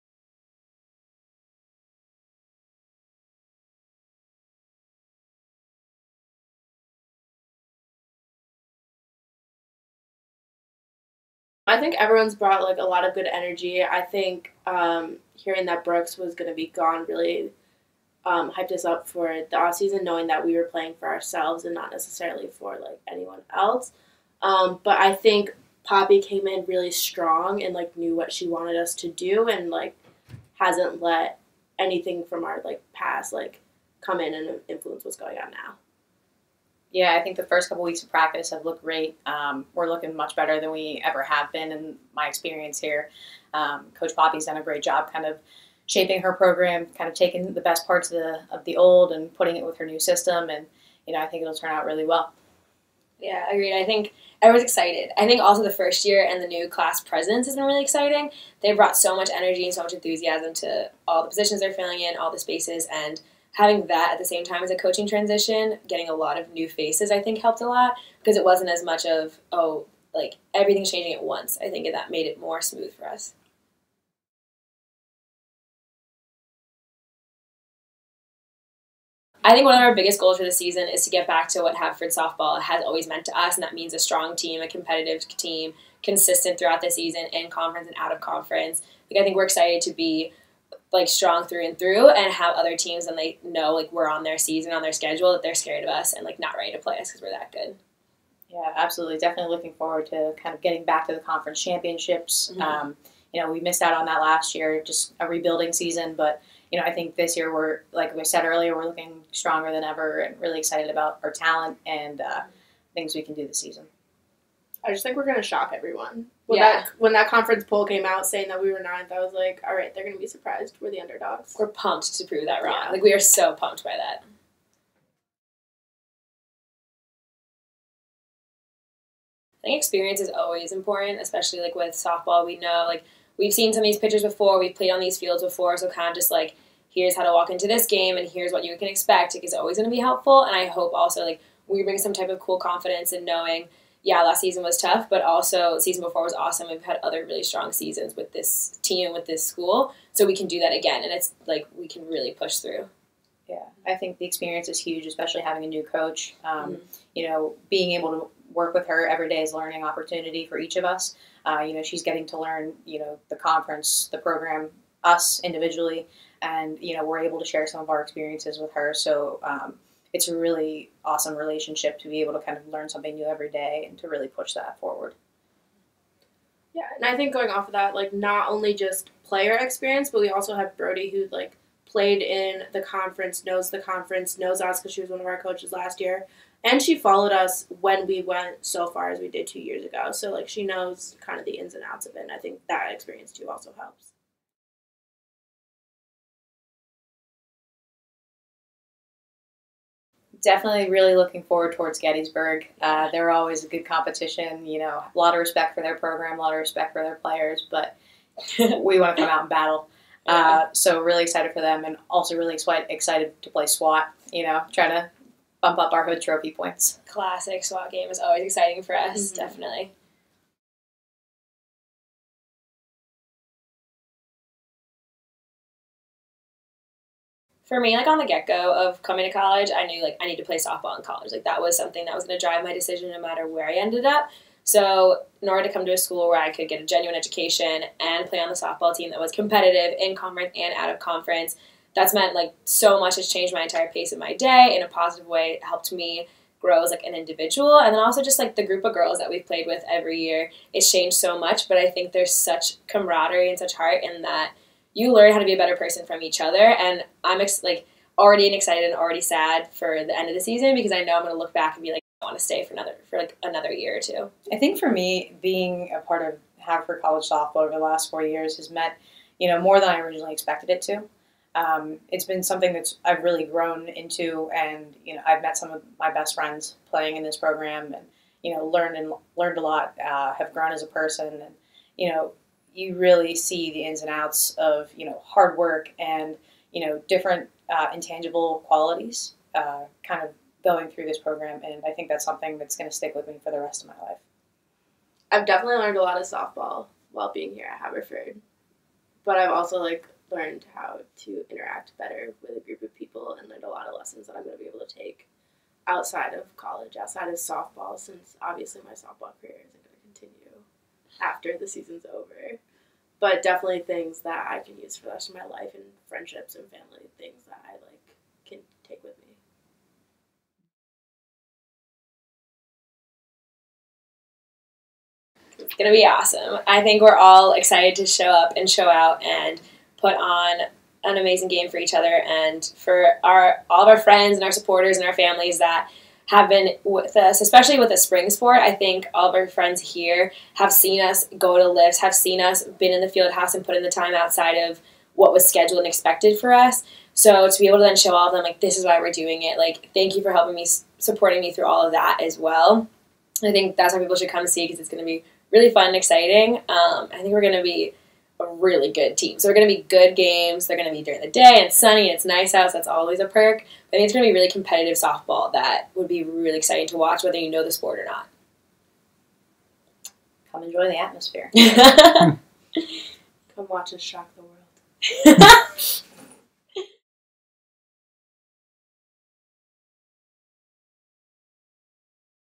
Softball 2018 Senior Preseason Interviews
Hear from the Haverford College Class of 2018 as they get set for the 2018 season.